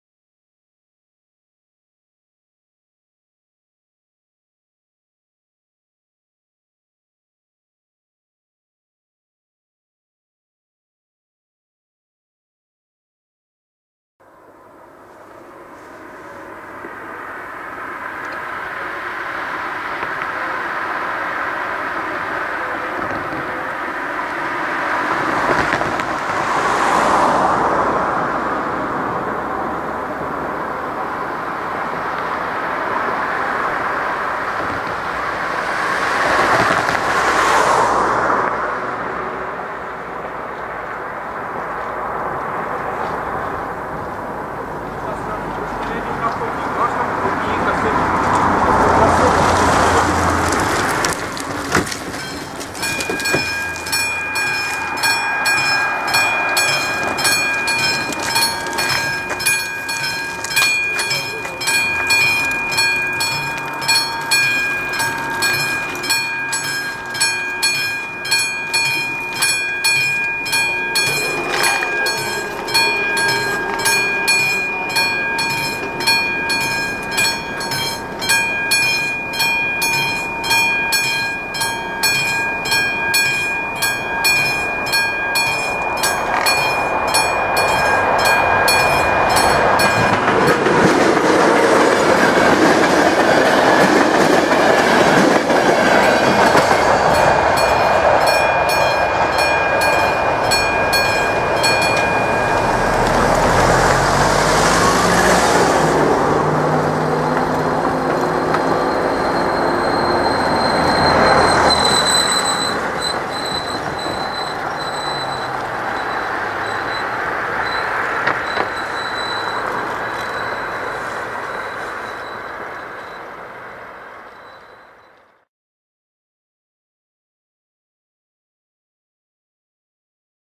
Železniční přejezd Hradec Králové [Kydlinovská] - 8.3.2014
Železniční přejezd na tomto videu je vybavený závorami, reflexními kříži a výstražníky AŽD 71 bez pozitivního světla, s klasickým zvonkem. Nachází se v ulici Kydlinovská nedaleko stanice Hradec Králové hlavní nádraží. Přes přejezd projíždí osobní vlak 5202 (Týniště nad Orlicí - Chlumec nad Cidlinou). Tento přejezd jsem již jednou točil, ovšem z opačné strany a společnost mi tenkrát dělal uživatel Michal Veselka. Nyní jsem jej točil s uživatelem Martinacek96, kterému děkuji za super den. Bohužel je video točeno proti slunci. Natočeno 8.3.2014 videokamerou Panasonic HC-V100.